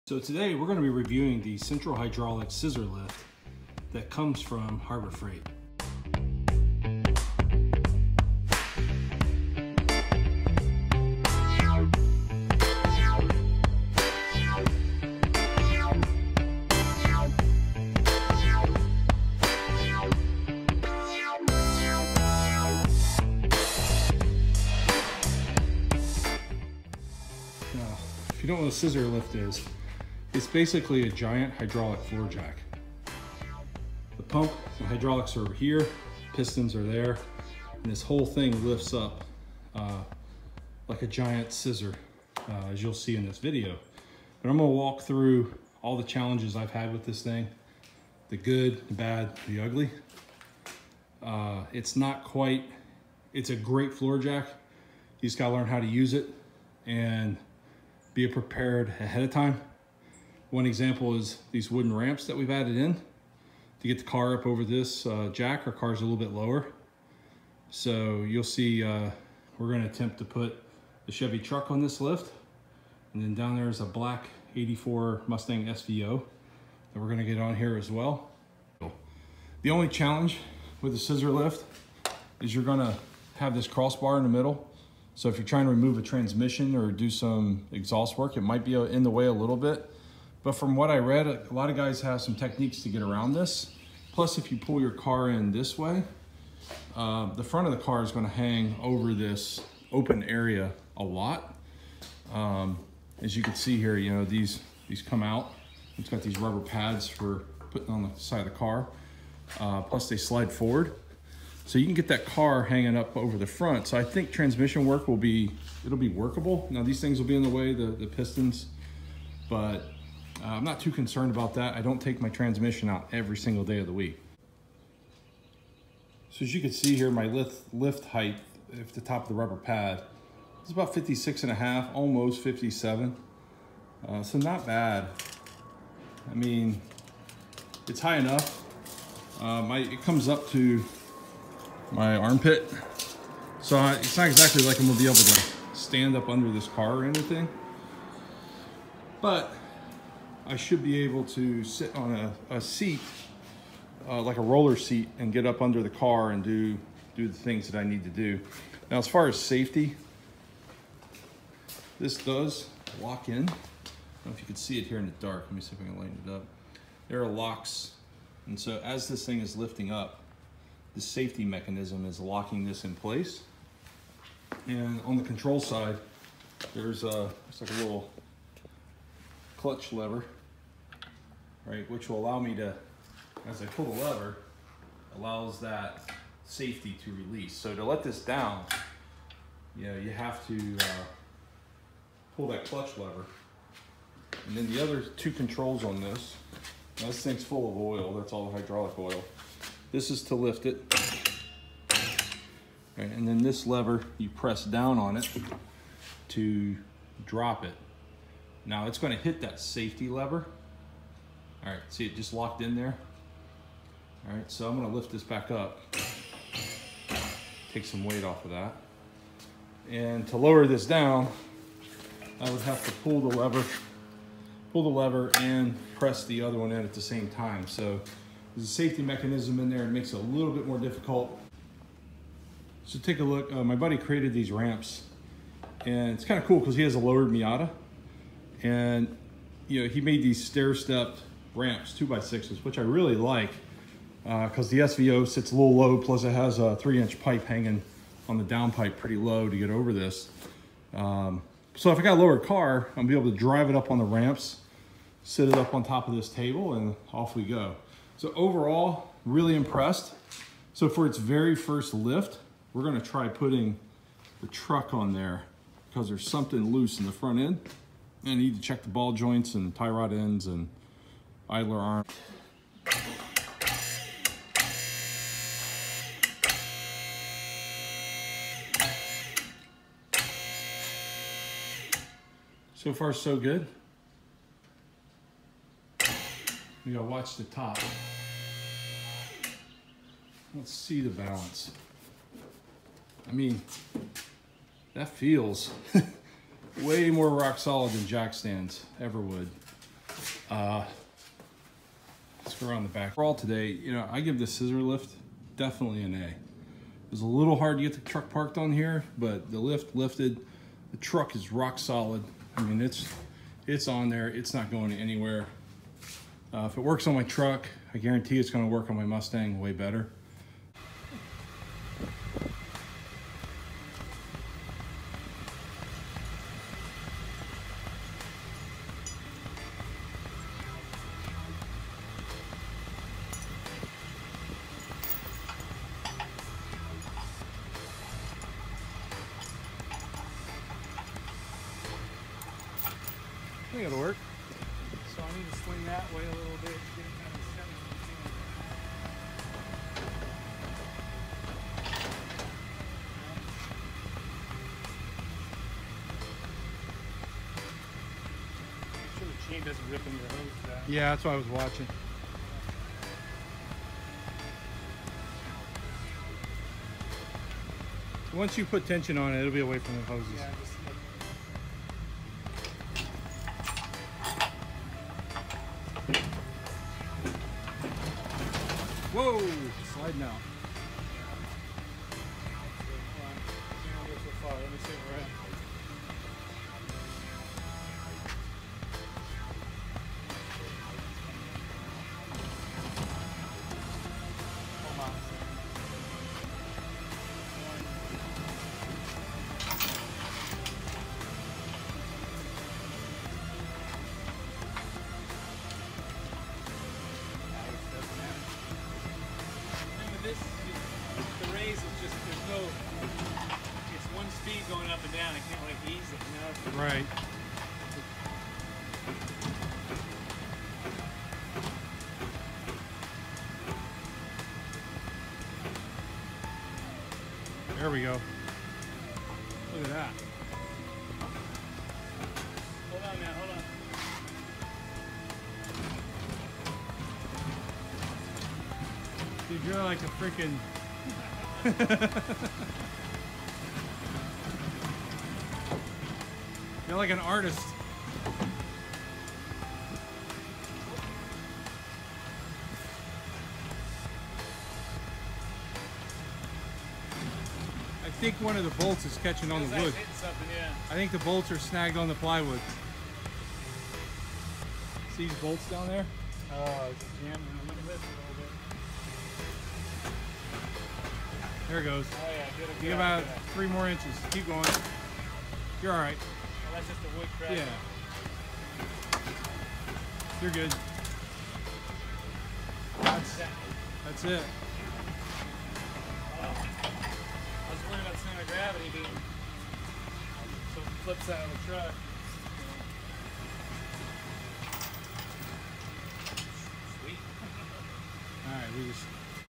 Harbor Freight Central Hydraulics Scissor Lift 91315 Review and lifting a 6000 lbs GMC truck
Harbor Freight Central Hydraulics Scissor Lift 91315 Review and demo lifting a 6000 pound GMC truck